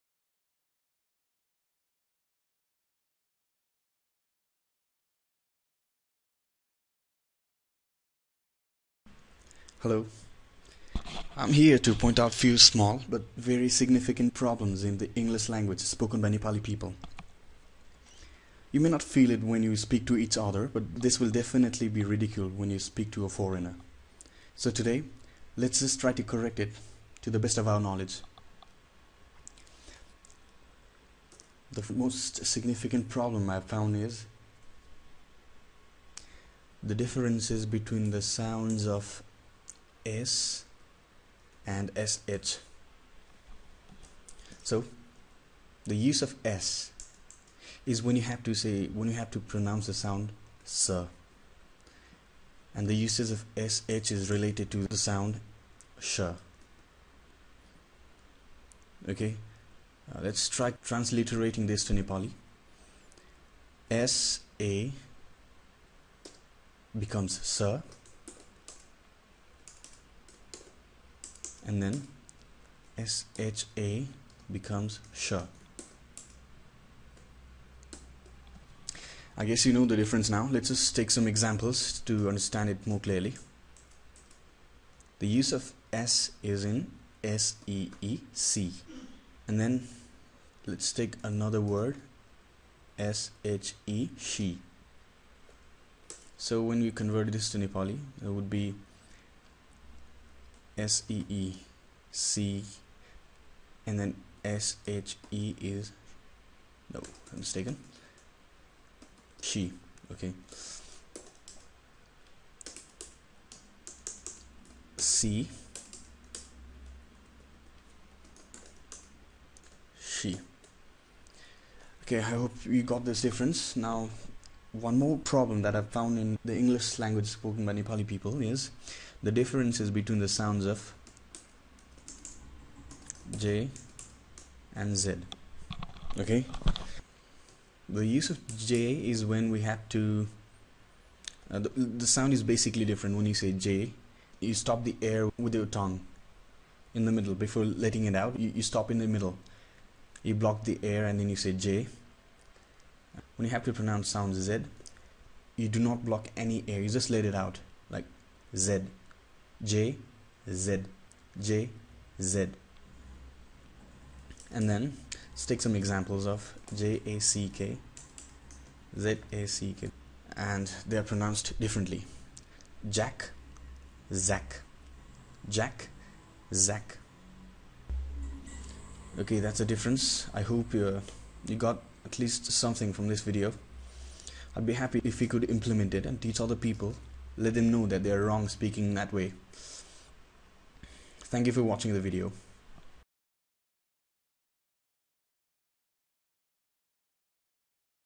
Nepali common mistakes Nepali-English mistakes Language translation nepali english
Video is best for general mistakes in J Z and  s  anb  Sha mistakes.
Correct it by learning. Dictionary is another solution
Keep learning on searching video.Thanks for guy for correction some mistakes. :)